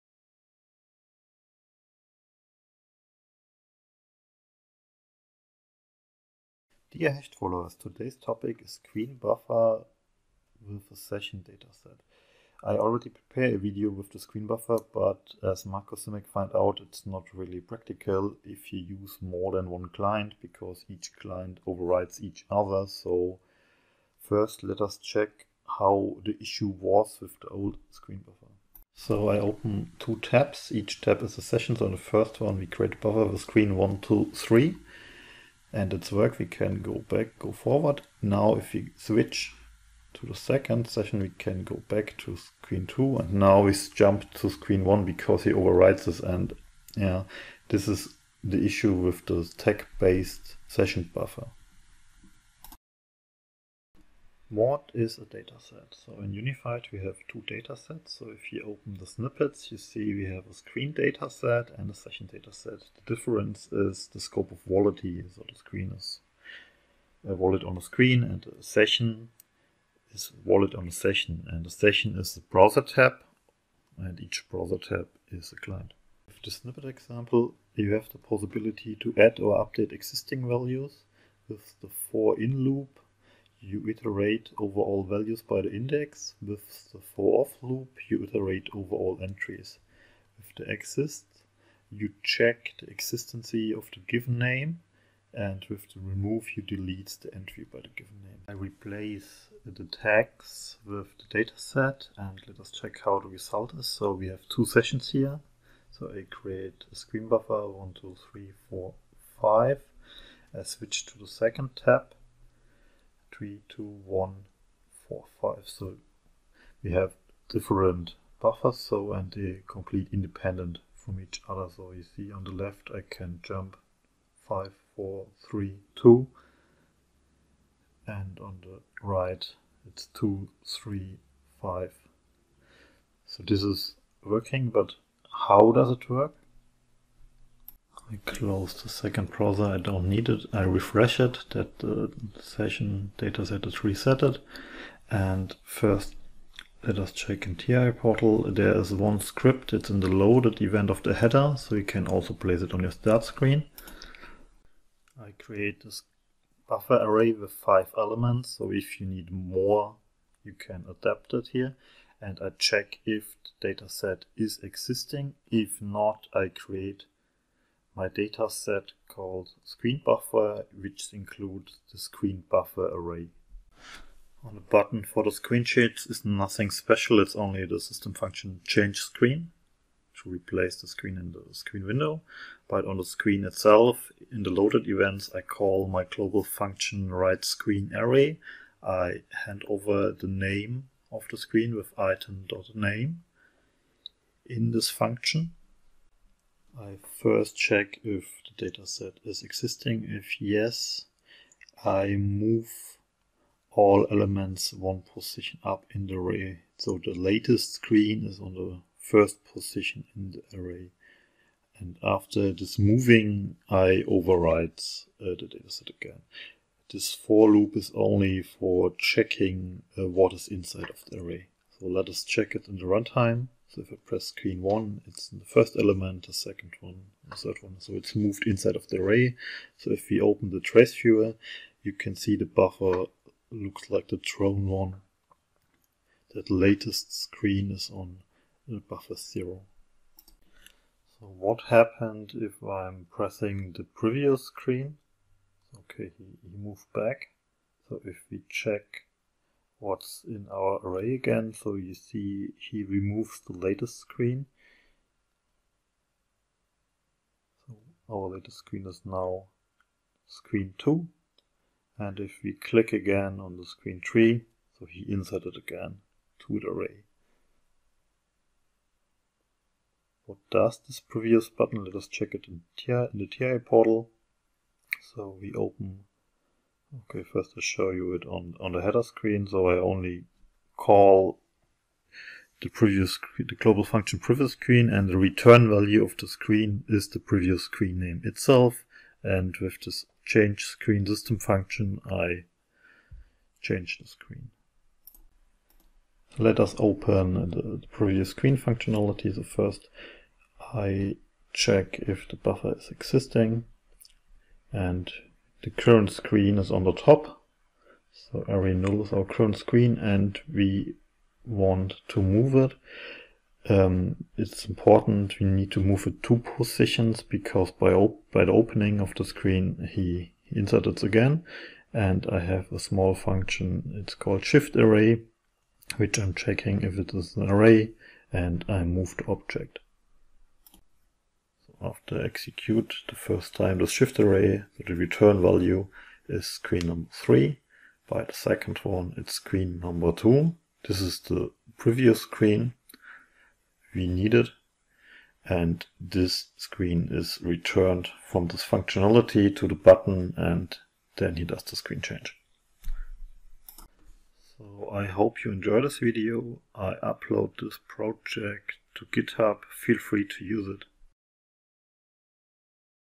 WinCC Unified V16: create your client independent Screen buffer with a DataSet
In the existing screen buffer Video I use tags to store the bufferd screens. The issues is, it work only with one client

In this Video I use a session based DataSet witch is for each client session unique. So you have a easy navigation to the previous screen

00:14 exisitng issue with the tag based screen buffer
00:54 what is a DataSet
02:24 How does it work